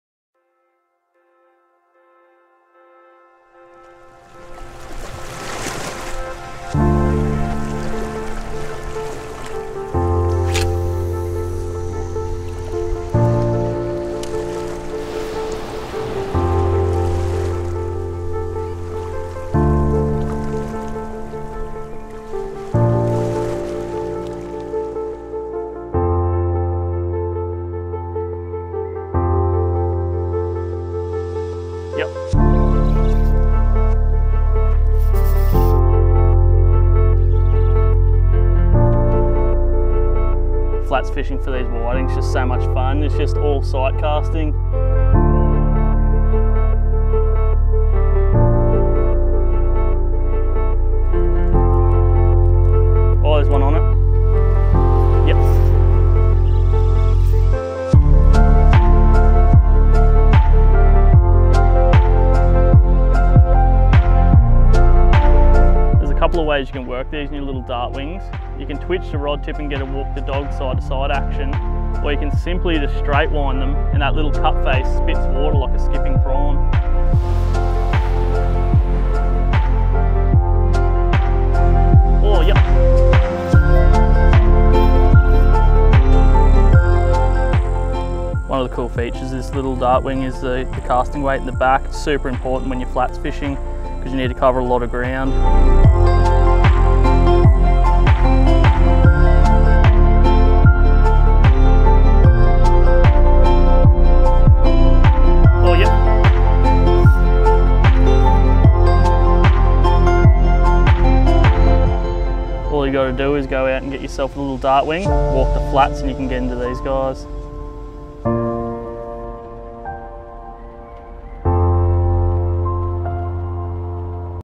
Nomad Dartwing 70 - Whiting on Surface
Dartwing 70 mimics a skipping Prawn to a tee. A great presentation for surface Whiting, the Dartwing also casts a mile. Watch the clip for great surface Whiting fishing.